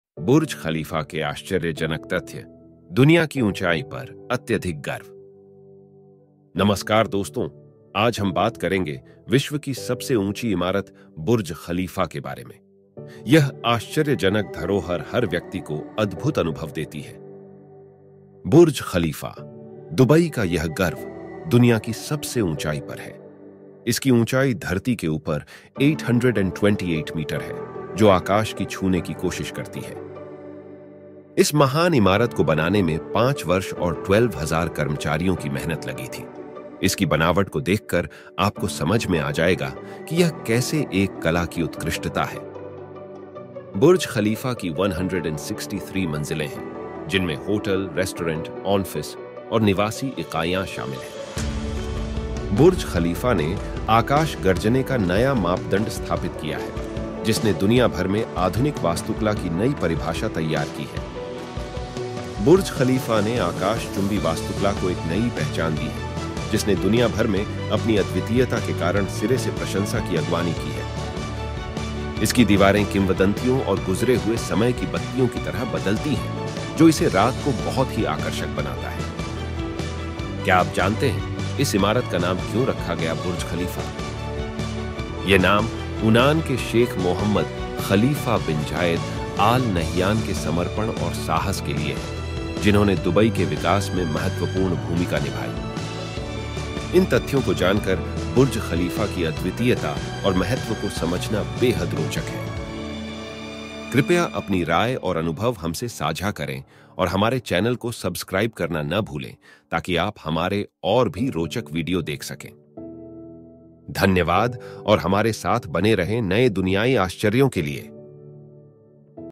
Discover the Hidden Truths of Burj Khalifa | Hindi
नमस्कार दोस्तों! आपका स्वागत है हमारे YouTube चैनल पर। आज हम एक आश्चर्यजनक सफर पर जा रहे हैं, दुनिया की सबसे ऊँची इमारत - बुर्ज ख़लीफ़ा के साथ।

पहला तथ्य, बुर्ज ख़लीफ़ा की अद्वितीय ऊँचाई के बारे में है।

इस इमारत की कुल ऊँचाई 828 मीटर (2,722 फुट) है, जिससे यह दुनिया की सबसे ऊँची इमारत बनती है।

बुर्ज ख़लीफ़ा का अद्वितीय डिज़ाइन भी एक अद्वितीय तथ्य है।

इसकी दिवारें किंवदंतियों और गुज़रे हुए समय की बत्तियों की तरह बदलती हैं, जो इसे रात को बहुत ही आकर्षक बनाता है।

क्या आप जानते हैं इस इमारत का नाम क्यों रखा गया बुर्ज ख़लीफ़ा?

यह नाम उनान के शेख मोहम्मद ख़लीफ़ा बिन ज़ायेद आल नह्यान के समर्पण और साहस के लिए है, जिन्होंने दुबई के विकास में महत्वपूर्ण भूमिका निभाई।

अब हम कुछ रोचक छोटे ज्ञान के बारे में बात करेंगे!

बुर्ज ख़लीफ़ा में एक अत्यधिक मूल्यवान दाम वाला एक बोन्ड किसी भी स्थान पर तरक्की करने के लिए डीप फाउंडेशन के रूप में बढ़ाया गया है!